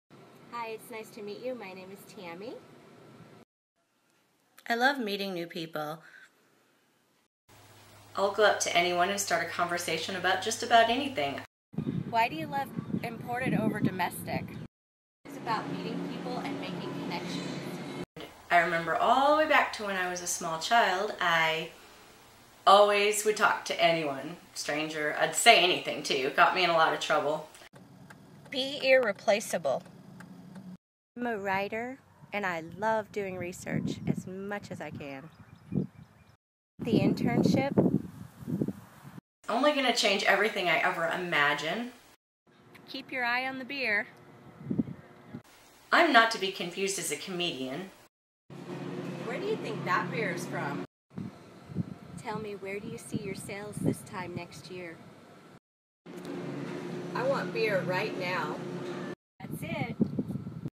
Tammy Rae
In applying for an internship they asked me to make an 'ode to beer' type video. To me beer is meeting new people; its where people come together and celebrate.